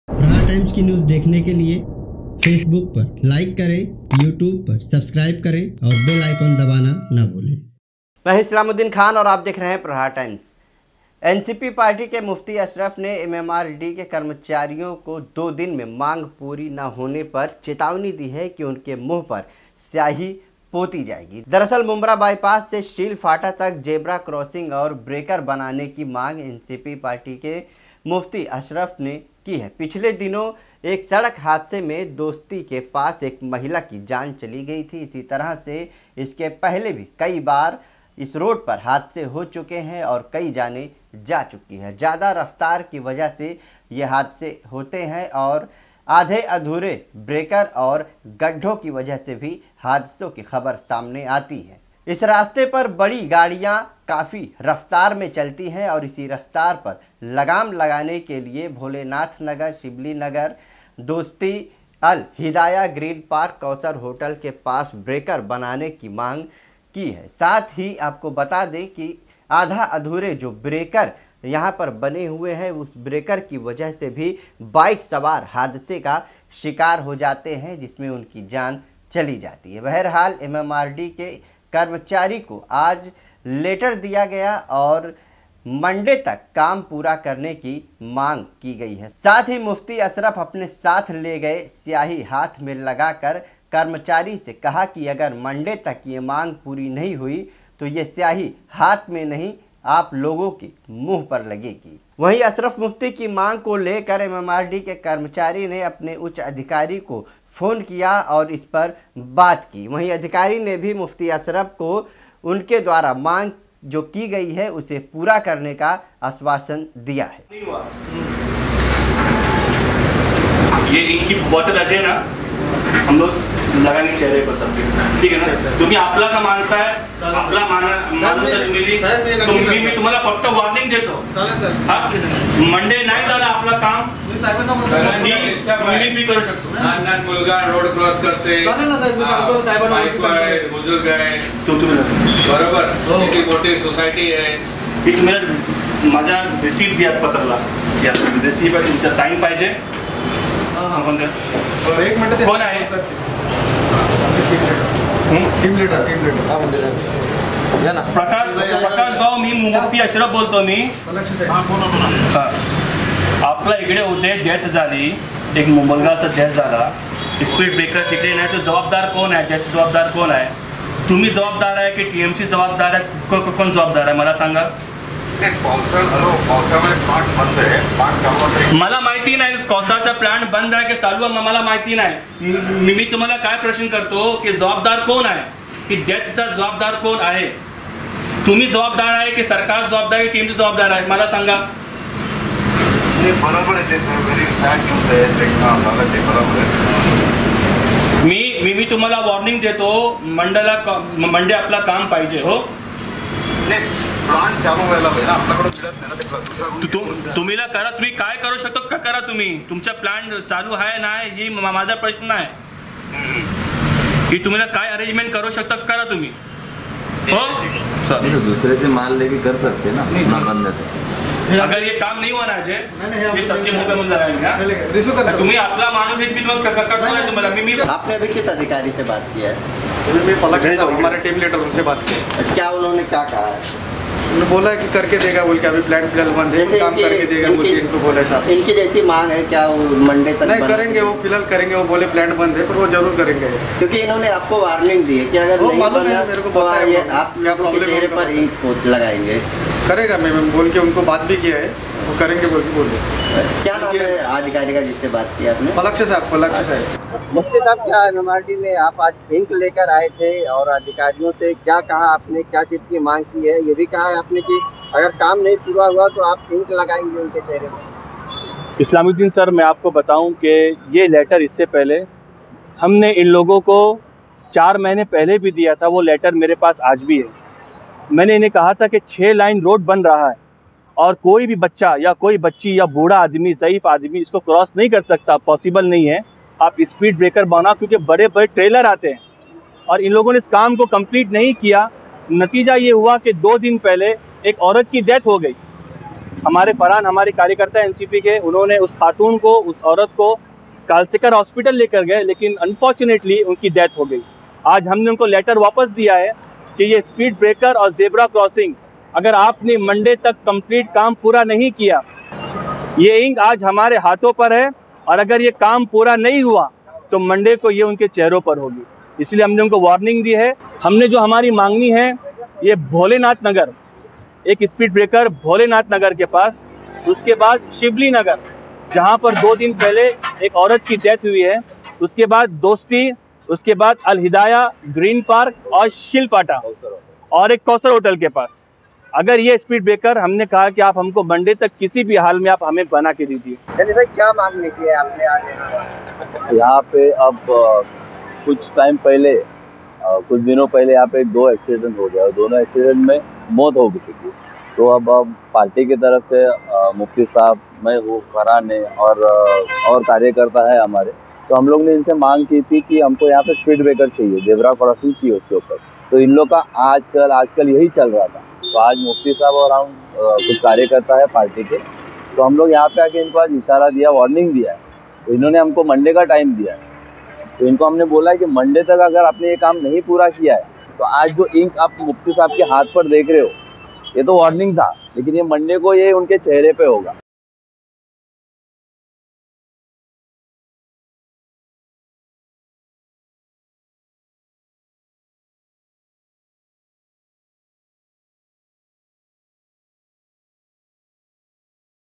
NCP के पदाधिकारी ने कर्मचारी के मुह पर स्याही लगने की चेतावनी दी || Islamuddin Khan
NCP के पदाधिकारि ने MMRDA कर्मचारि के मुंह पर अगर इनकी मांग नही पूरी हुई तो स्याही पोतेंगे जेब्रा क्रॉसिंग और स्पीड ब्रेकर बनाने की मांग की है